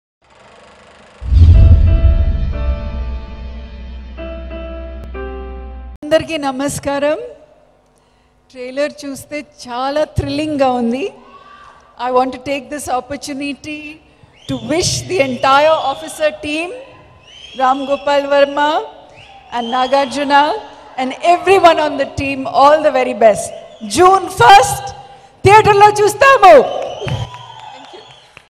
Amala Speech @ OFFICER Movie Audio Launch | RGV | Nagarjuna | Myra Sareen
Watch OFFICER Movie Audio Launch | RGV | Nagarjuna | Myra Sareen
Officer 2018 Latest Telugu movie ft. Nagarjuna and Myra Sareen. Directed by RGV and Music composed by Ravi Shankar. Produced by Ram Gopal Varma and Sudheer Chandra under A Company Production.